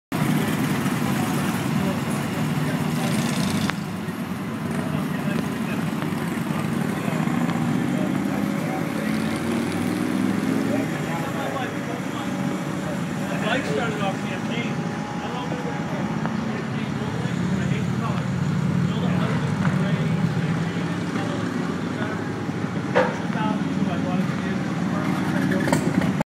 Fire Fighter tribute bike. LEDs with Fire Truck part 2
Fire Fighter tribute bike. With LEDs and scale Fire Truck. Seen at Laconia Bike Week 11'